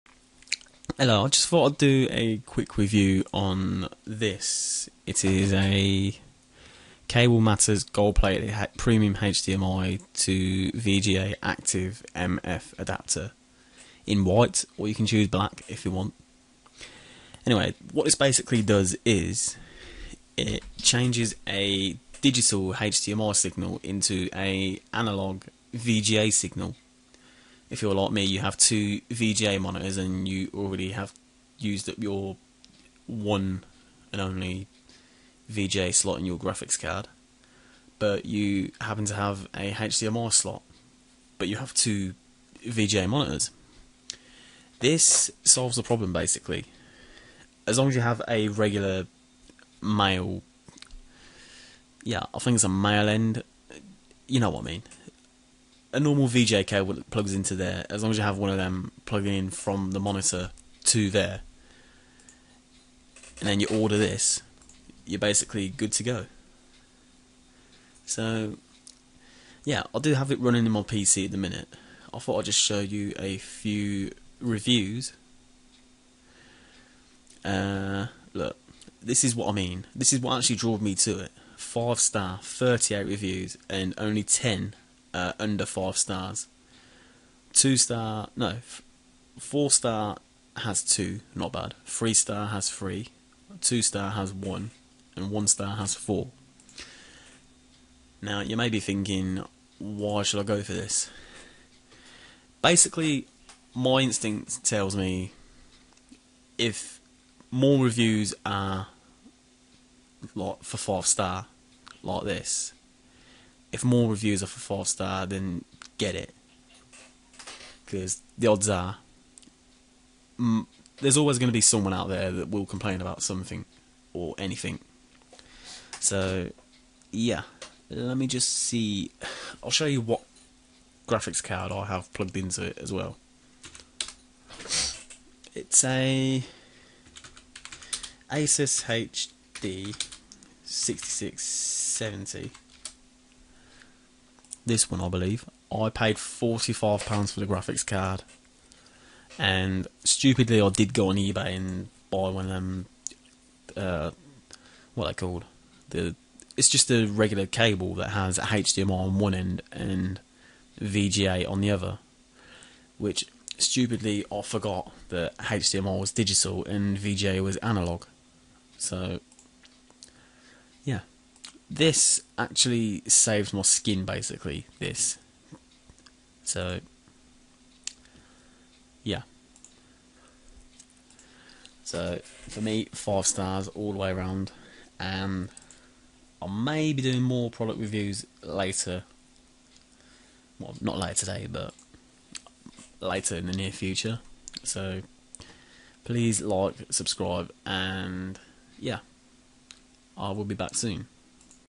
Cable Matters Gold Plated Premium HDMI to VGA Active M-F Adapter REVIEW
this will convert any hdmi (digital signal) into a vga (analog signal)